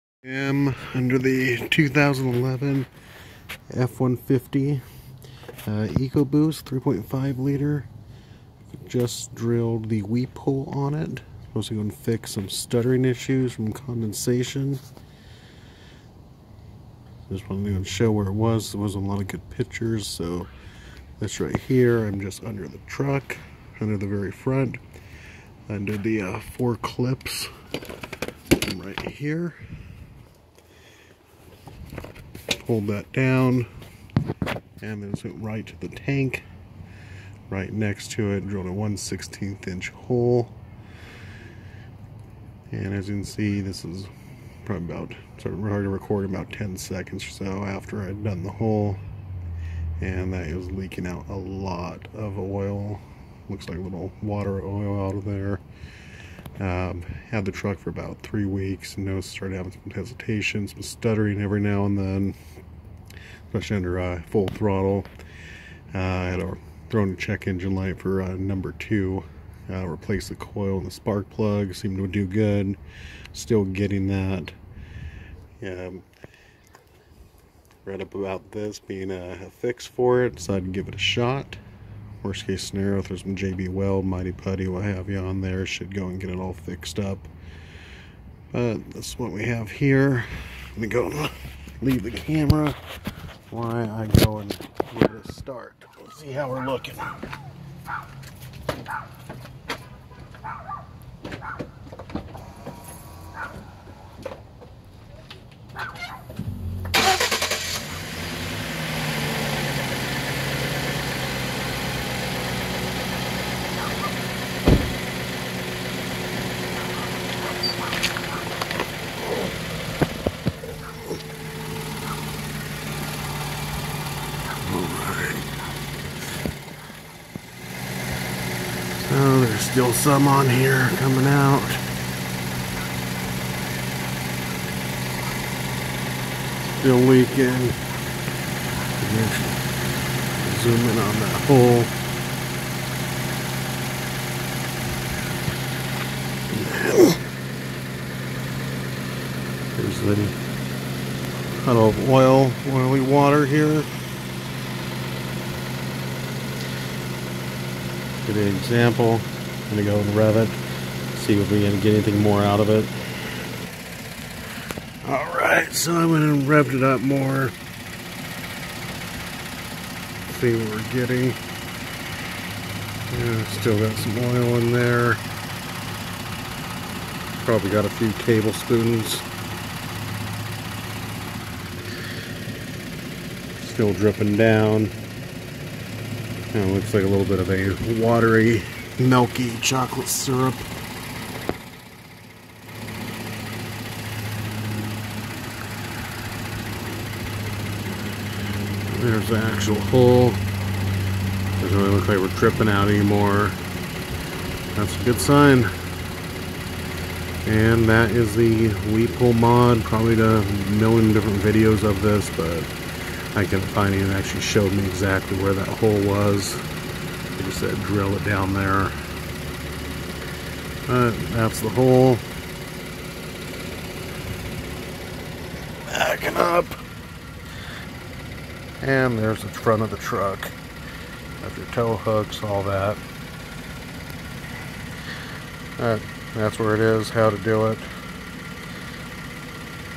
2011 F150 Ecoboost Intercooler weep hole drilling mod
This mod is meant to remedy the shuddering idle and acceleration in our trucks. In some cases it causes misfires.
This is caused by oil and condensation (worse if you're in a humid or colder area) pooling in the intercooler and getting sucked into the intake track.
This is part of the problem. The other part is the PCV system, which is remedied by installing a catch can.
In this video I will show you exactly where the hole is to be drilled. It should be drilled with a 1/16" drill bit in the lowest part of the intercooler. It is easily accessible and the mod only takes a few minutes.
There have been concerns about purposely adding a leak in a pressurized system. This small hole is very minor and does not negatively affect performance or power.
It will however smooth out the engine if your problem is caused by oil and condensation getting in your intake track.

So after driving it I made the two observations,
1: The shudder is gone. It is so much smoother.
2: My boost gauge now goes up to 13.5psi maximum. Prior it would peak up to 16psi. I have not noticed a loss of power. I'm guessing the drop is that now the turbos are now breathing better and is not as restrictive.